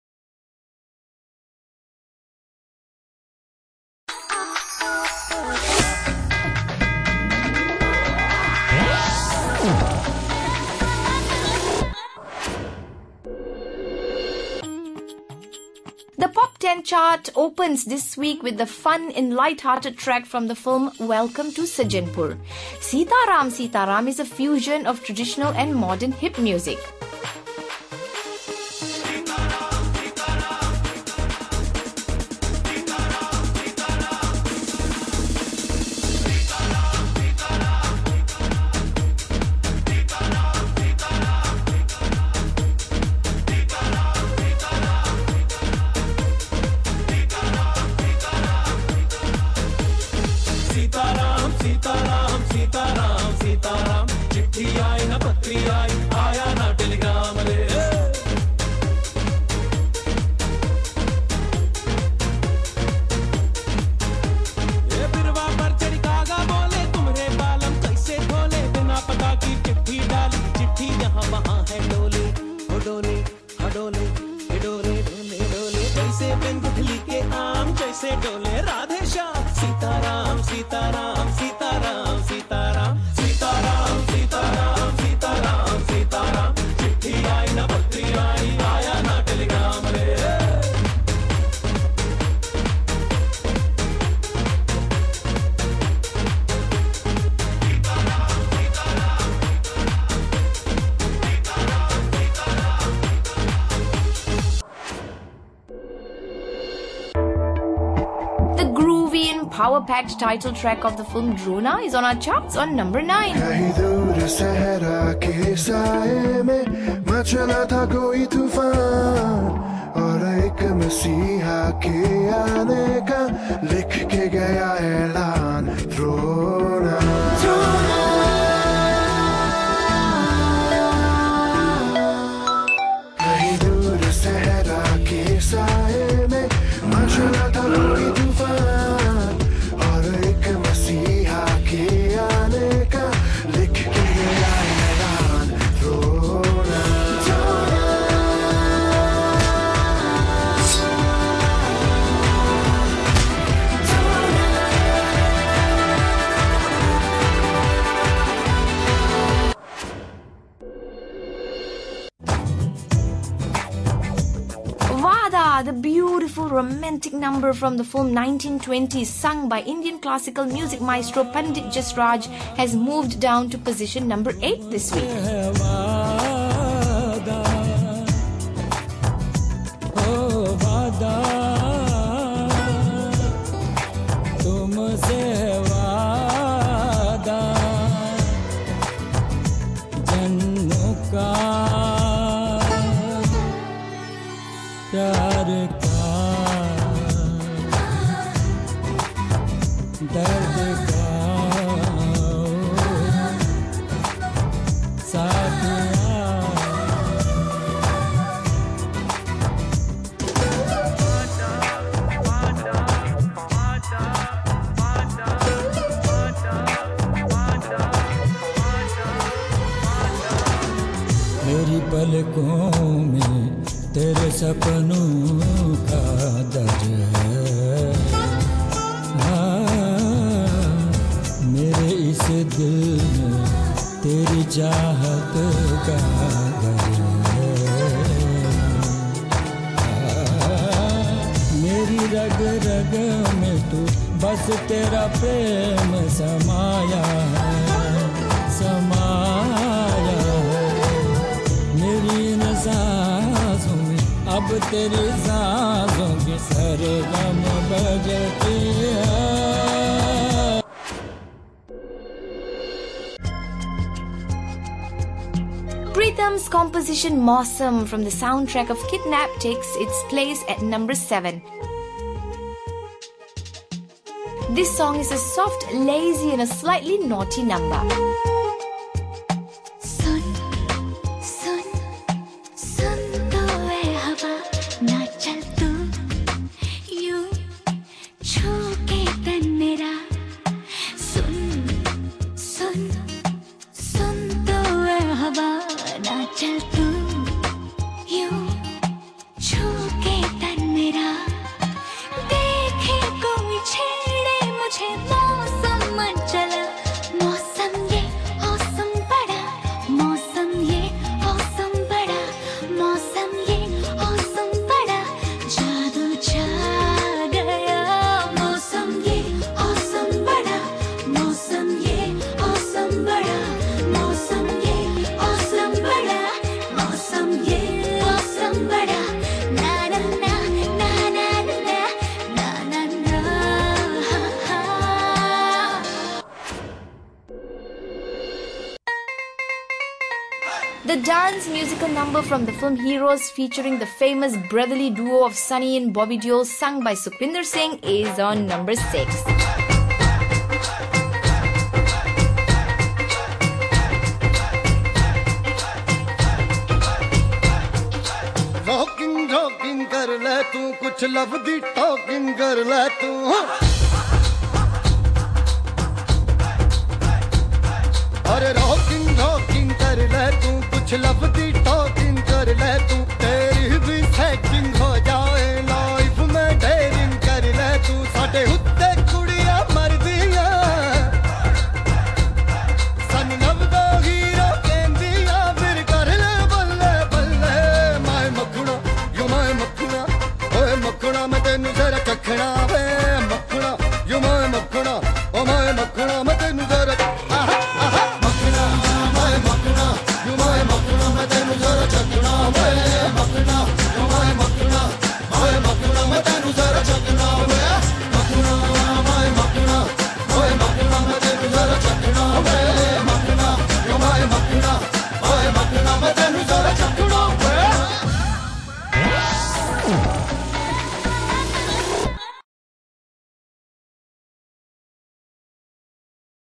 Top Ten Part 1 on MyTime vol 25
Catch the musical struggle between coolest Bollywood songs and find out for your self which ones have rolled down or climbed up the weekly chart.

We have successfully launched IPTV on mobile with (MyTime) DIGI in Malaysia, soon to be launched in Dubai, Bangkok, China & India.